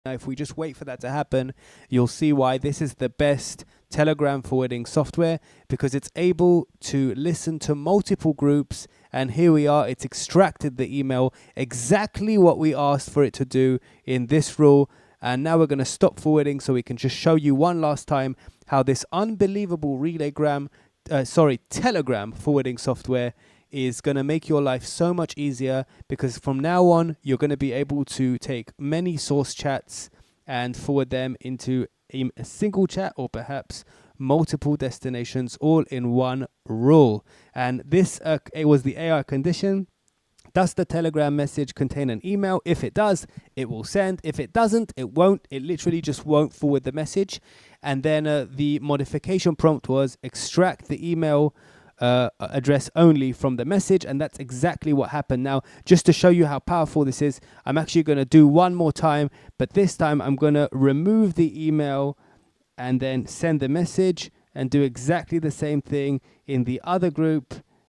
Best Telegram Forwarding Software: Simple Setup Guide!
We're going to showcase the BEST Telegram forwarding software! We'll demonstrate how to effortlessly forward from multiple groups to single or multiple destinations with AI email extraction, making your life easier. See how simple it is to set up!